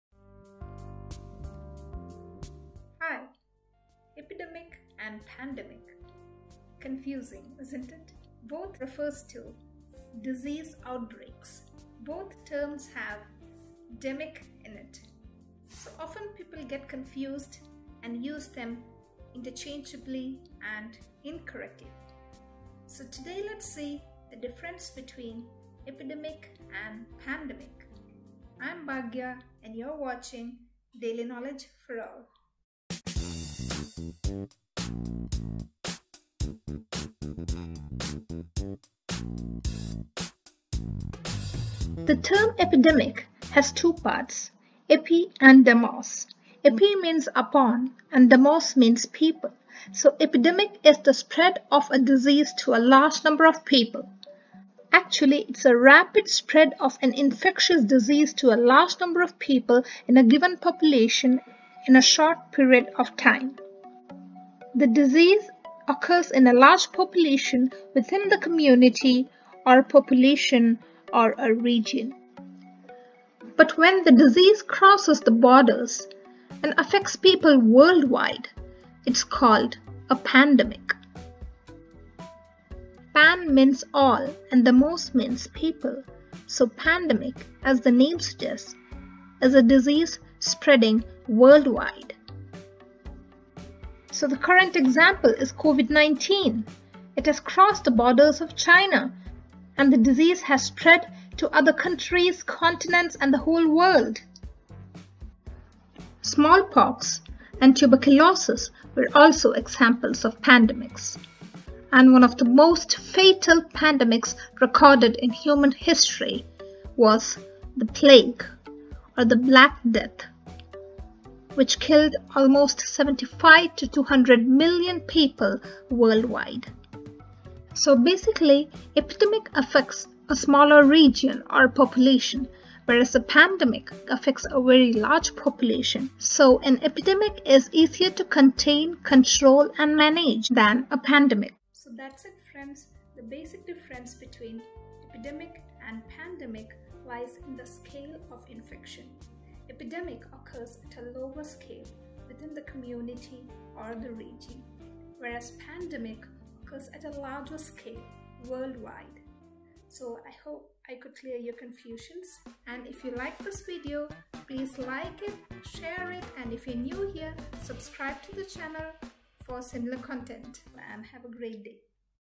Epidemic Vs Pandemic ! What is the difference between the two terms🙄 ConfusingTerms Explained@DK4all
We often hear these two terms..Epidemic and Pandemic..But what is exactly the difference..can it be used interchangeably? Watch the video to know the clear difference between the two terms..
Of course if you have anything to add on to what I have said, please drop it in the comments section below..
And I tried to troll my video for a change. I don't know exactly how to troll a video..but I tried..just check it out: